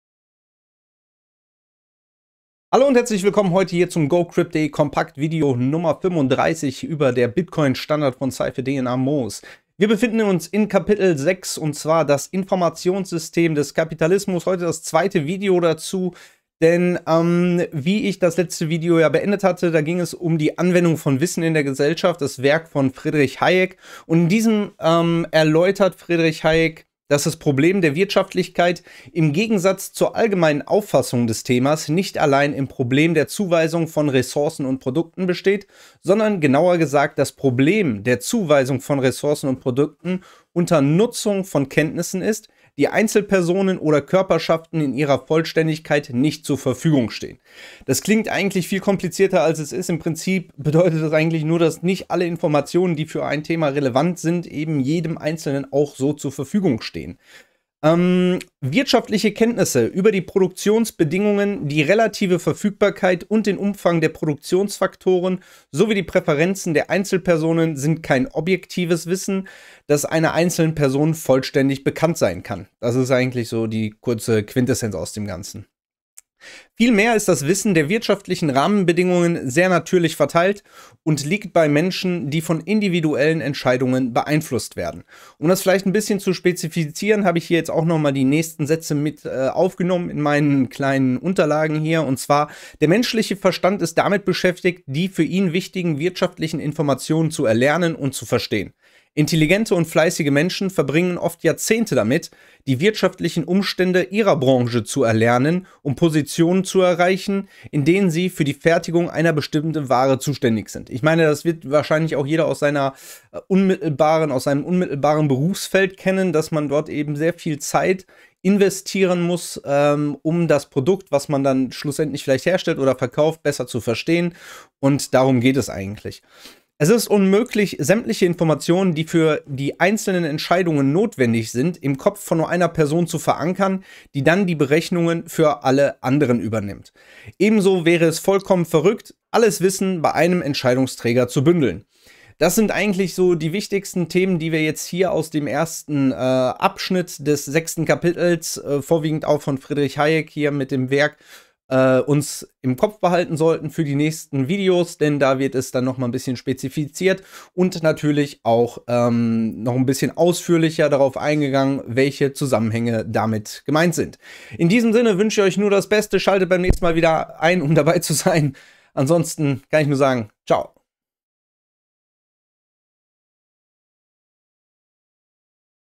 GoCryp.de - compact #35 - Kapitel 6 - Das Informationssystem des Kapitalismus - der bitcoin standard
In dieser compact Serie beschäftigen wir uns mit dem Buch "der bitcoin standard" von Saifedean Ammous.

Folge #35 erläutert die These von Friedrich Hayek und seinem Werk. Diese Folge gibt somit mehr Informationen zum aktuellen Thema.

▬▬▬ Timestamps ▬▬▬
0:00 - Intro & Vorwort
0:25 - Das Problem der Wirtschaftlichkeit
0:58 - Wirtschaftliche Kenntnisse sind kein objektives Wissen
1:18 - Das Wissen ist natürlich verteilt
1:30 - Weite Ausführung aus dem Werk von Friedrich Hayek: "Die Anwendung von Wissen in der Gesellschaft"